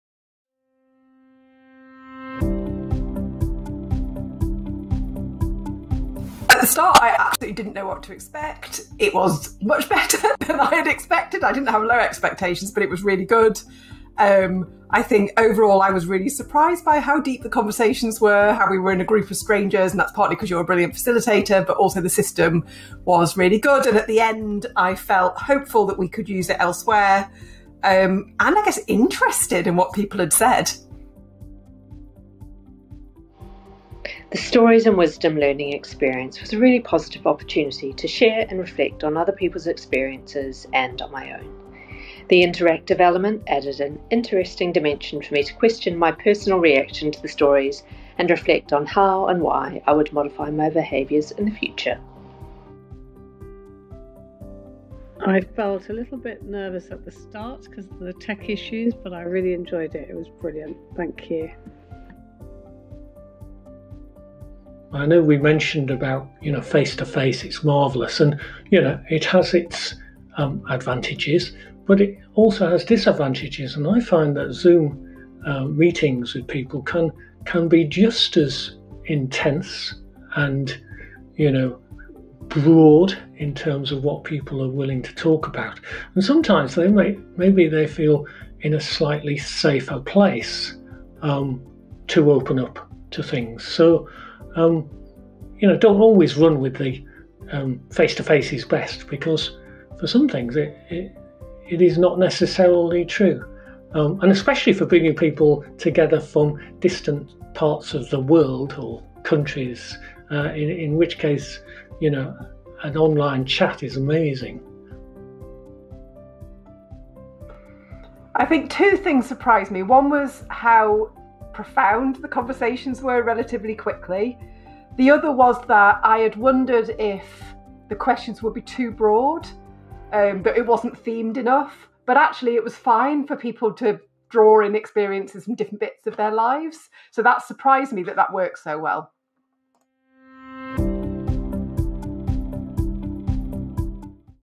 Cybility Stories - The Trustee Perspective
We teamed up with Penny Wilson, CEO at 'Getting on Board' and invited some trustees to participate in a session like no other.

The topic: Team and Culture.